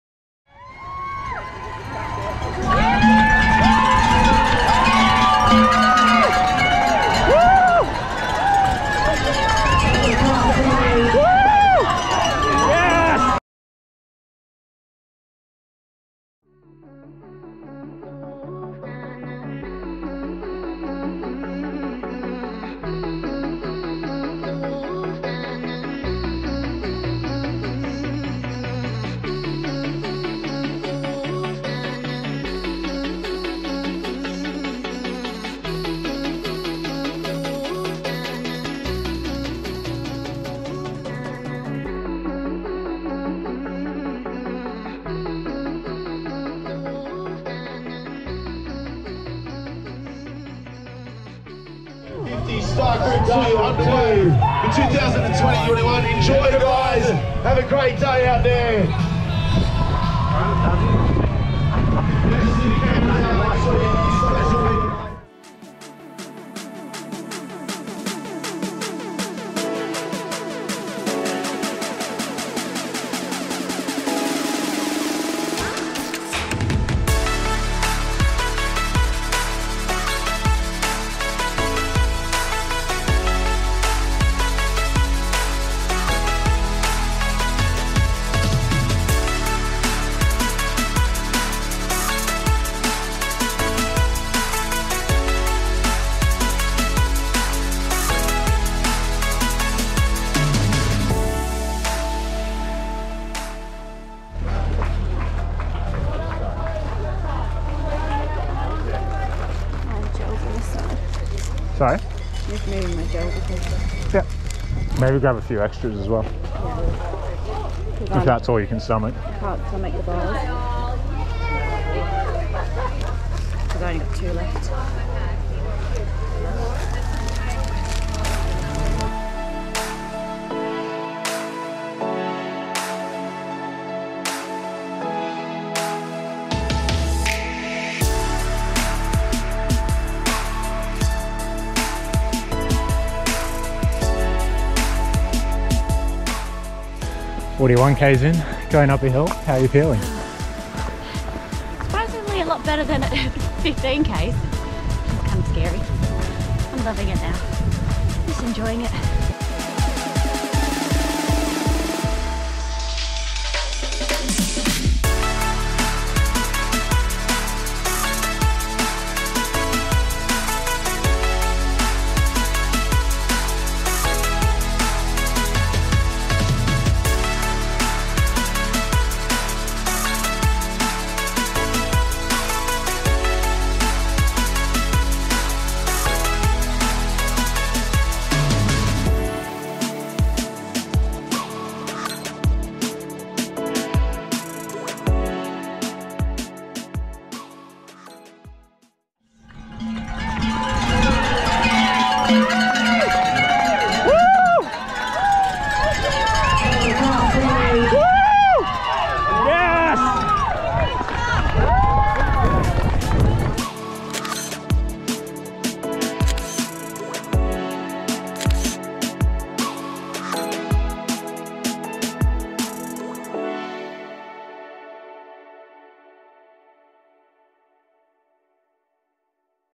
UTA50: Rachel's first Ultra | A Trail Running film | Ultra-Trail Australia 50km 2021
In 2021 Rachel decided to challenge herself to run her first ultra-marathon. For this she chose the Ultra-Trail Australia 50km (the UTA50) - A challenging course with over 2000m of elevation (much of that on stairs) set in the stunning Blue Mountains of Australia.

This is a short trail running film documenting some of that journey through the UTA50.